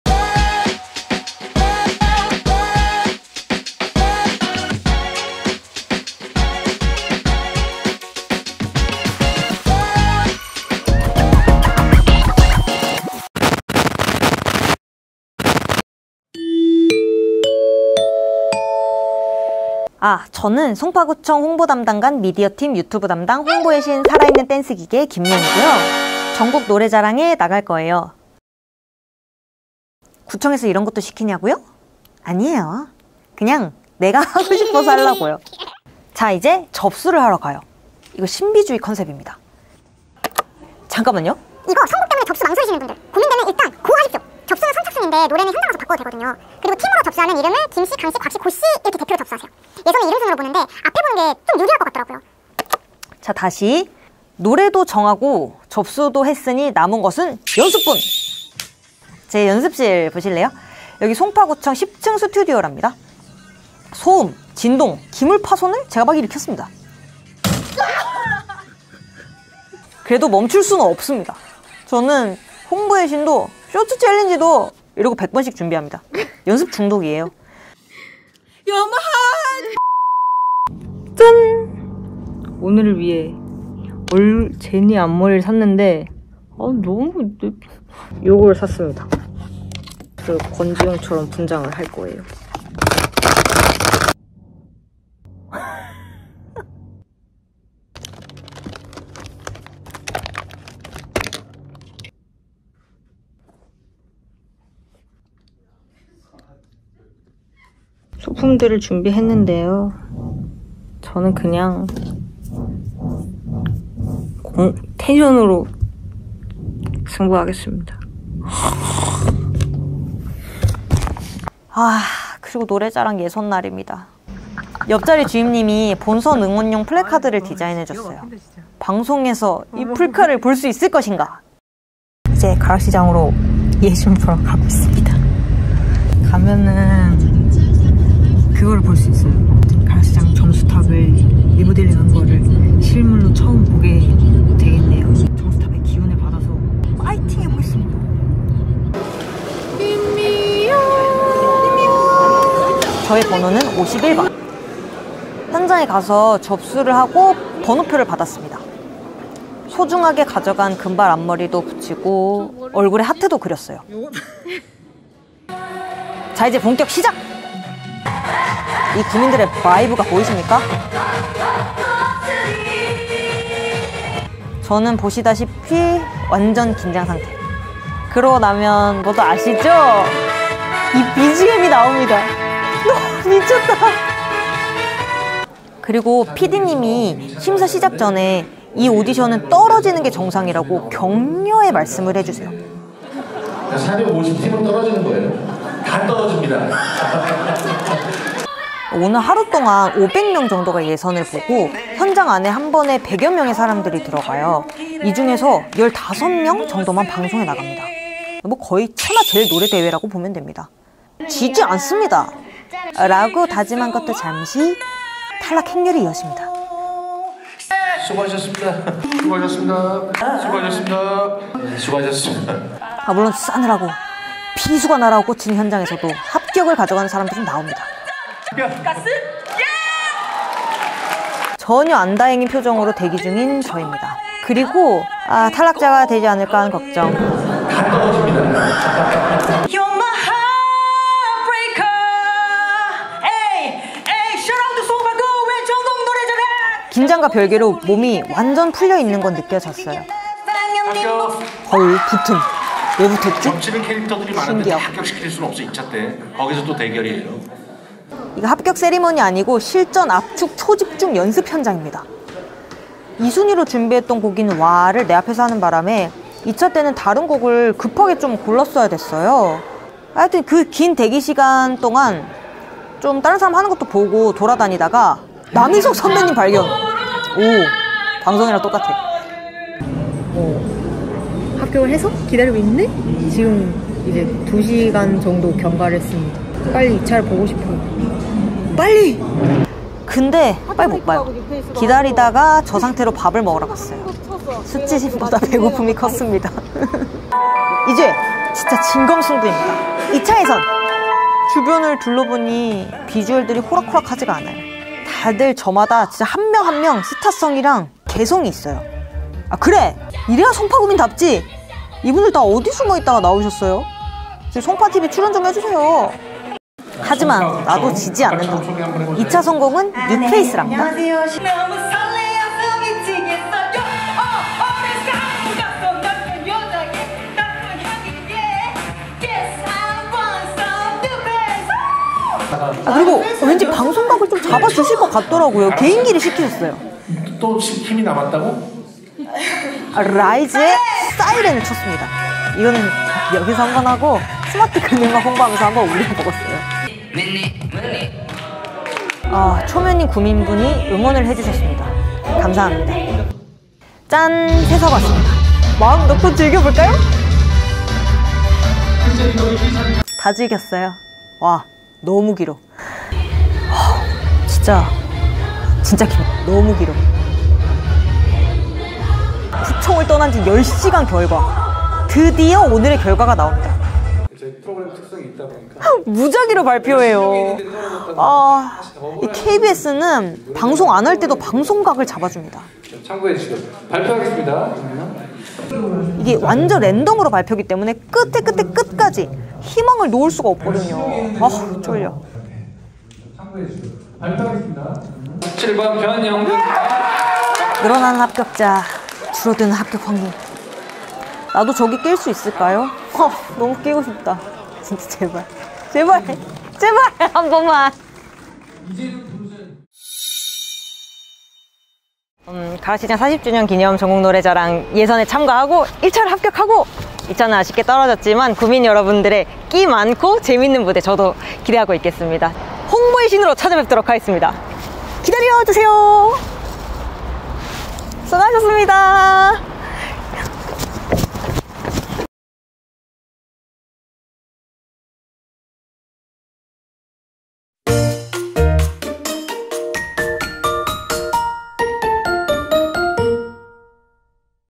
드디어 공중파 진출?! 전국노래자랑에 내가 나온다면~~🎶🎹 본선 진출 tip 방출!
홍보의 신 '김미연' 주무관의 KBS 전국 노래자랑 도전기!
그녀는 과연 어디까지 갔을 것인가?!
본선진출을 꿈꾸며 함께 고생하신 모든 분들 챔피언!